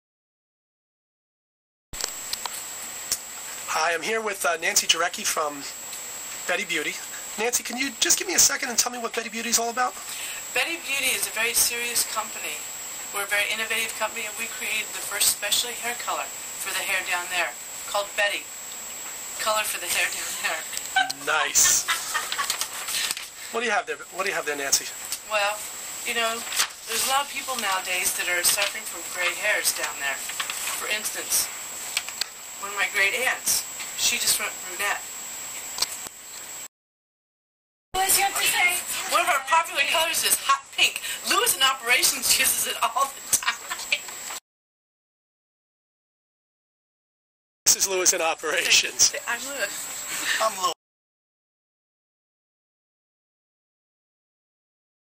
Pubic Hair Dye - Pink Hair Dye - BettyBeauty
Pubic hair dye for your Hair Down There.  Pink is the hottest selling pubic hair dye color. Find out more.  Betty Beauty